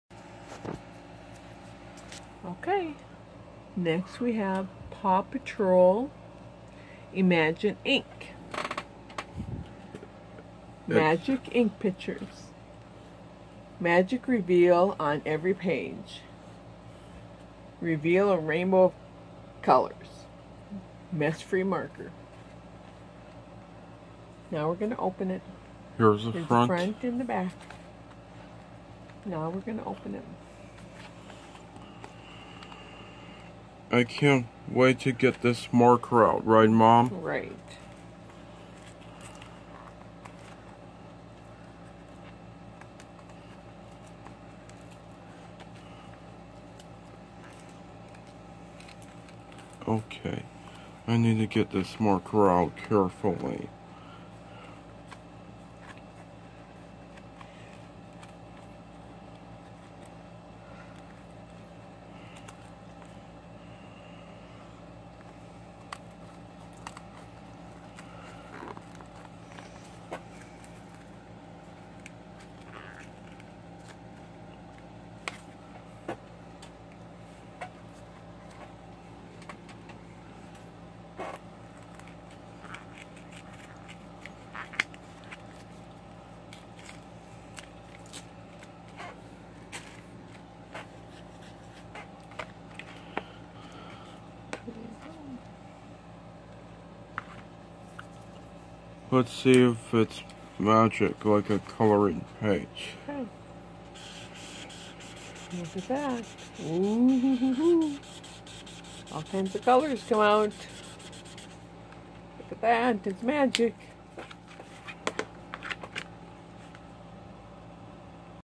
PAW Patrol - Imagine Ink Coloring Book Unboxing and Review
Here is an unboxing and review of PAW Patrol - Imagine Ink Coloring Book that I picked up from Target.

Recorded on June 16, 2021 using a Samsung Phone.